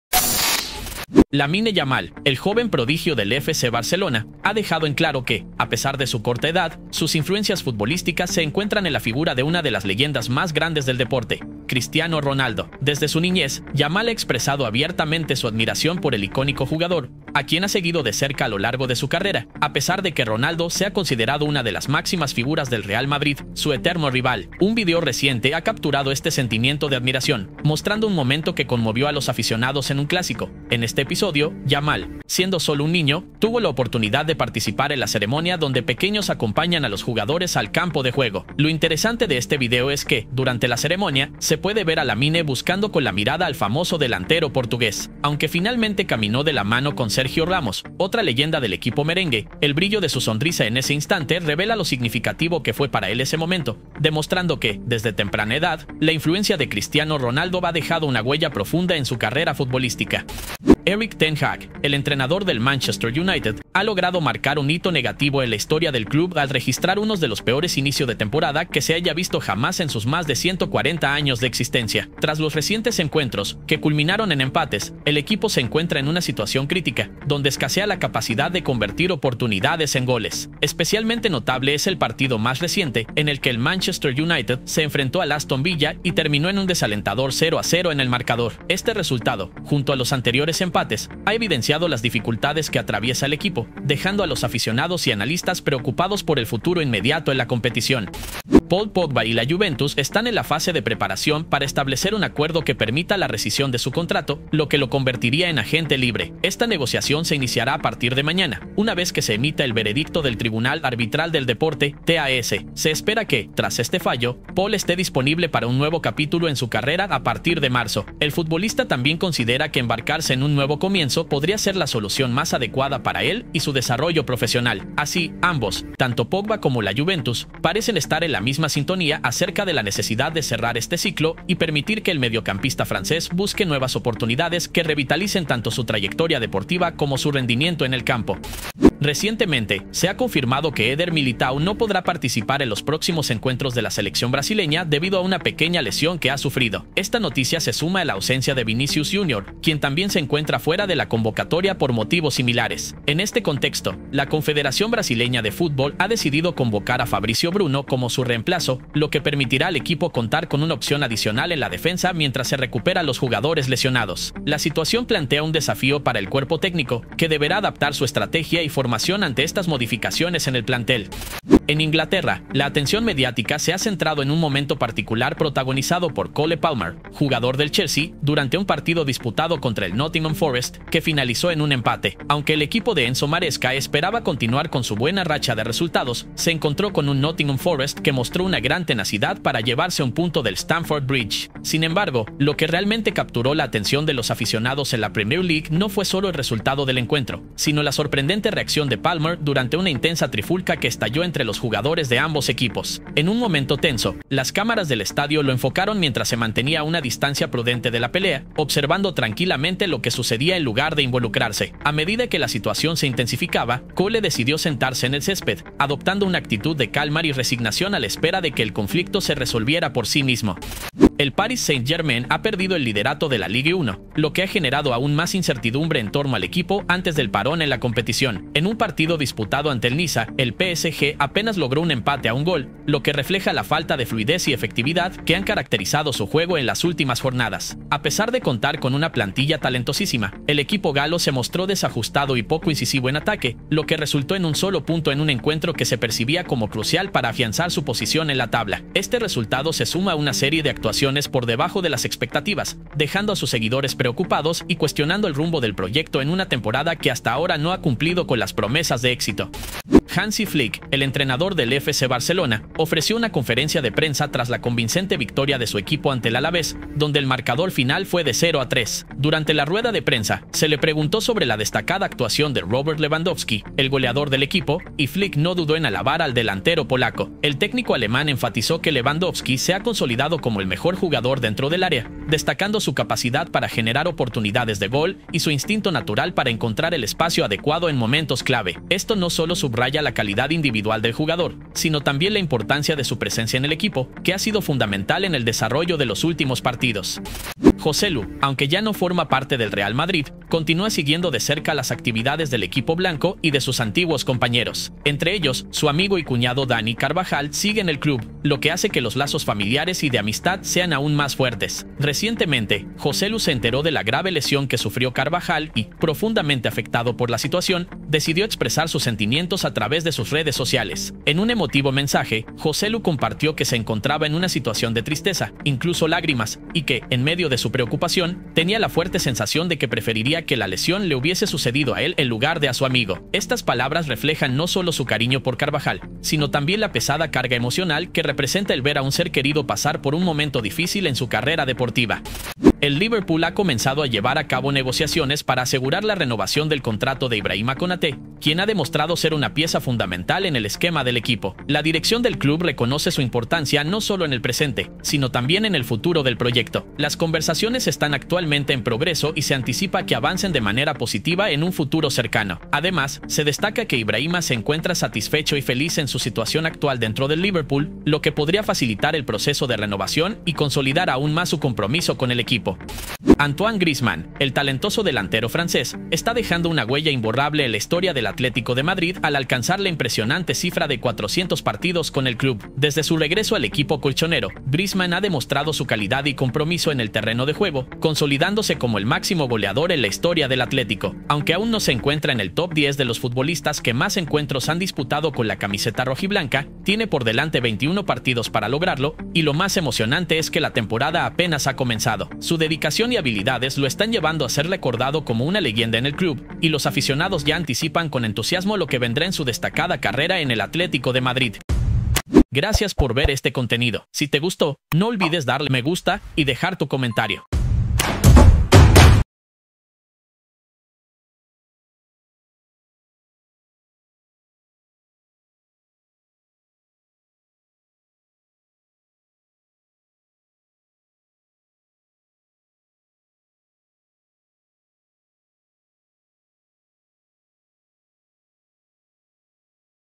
Yamal y el día que enloqueció por CRISTIANO RONALDO, Ten Hag con el peor inicio del UNITED
🧩 Gracias